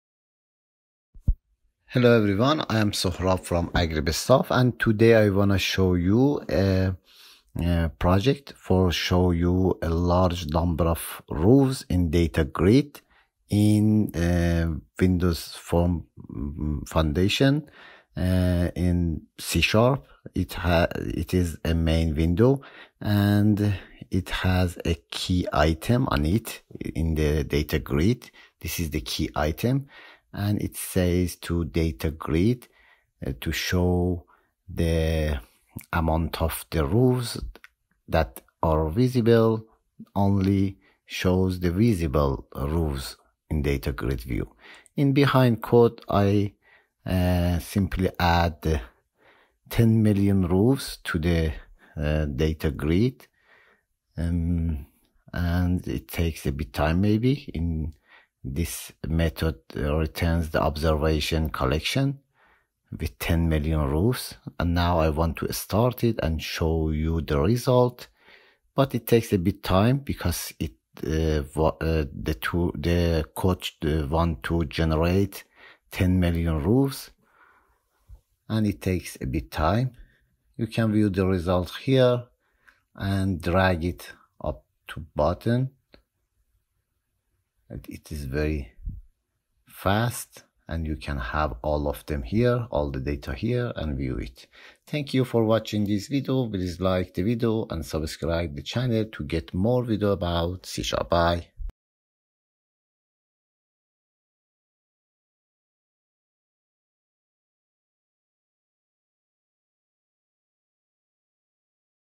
Make Your WPF DataGrid Handle 10M+ Rows EASILY!
How to display a large number of rows in a DataGrid
Join Sohrab from AgriMetSoft as he showcases a C# Windows Presentation Foundation (WPF) project focused on handling large datasets in a DataGrid. This video demonstrates the power of WPF's virtualization capabilities, where the DataGrid intelligently renders only the visible rows, leading to significant performance gains. I add 10 million rows in the background and then demonstrate the smooth and fast scrolling through this extensive data. Learn how WPF can effectively manage and display substantial amounts of information.